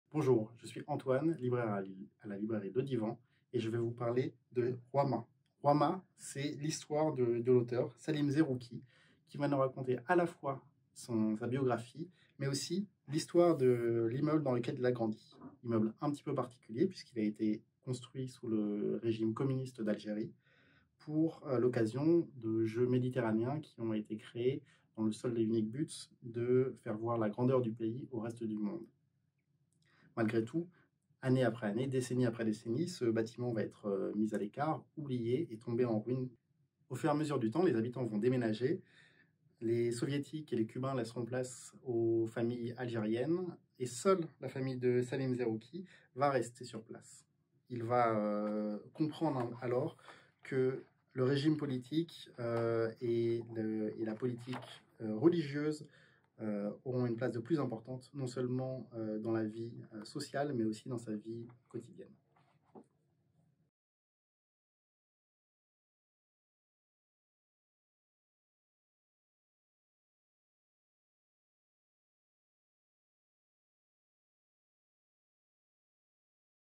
Que lire ? ♡ Antoine vous conseille 𝘙𝘸𝘢𝘮𝘢 de Salim Zerrouki
Antoine de la librairie le Divan partage ses lectures.

💖📚 Notre mot sur 𝘙𝘸𝘢𝘮𝘢 de Salim Zerrouki et publié aux éditions Dargaud